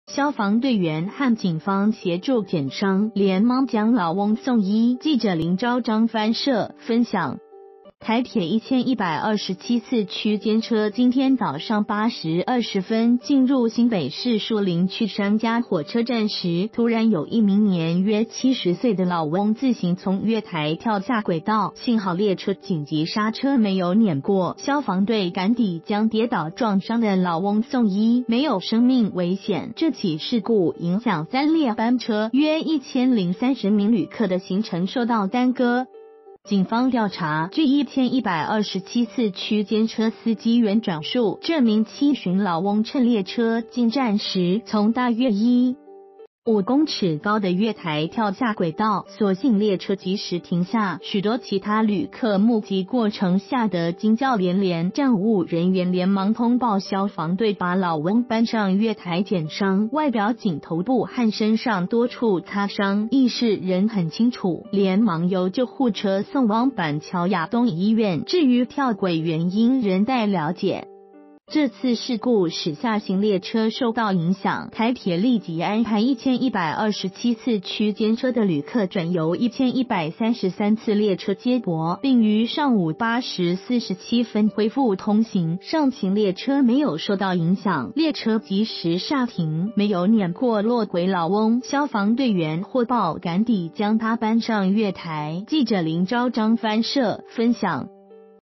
山佳站老翁突然跳軌 火車急煞幸未輾過
消防隊員和警方協助檢傷，連忙將老翁送醫。 記者林昭彰／翻攝　 分享

發表評論評論如下。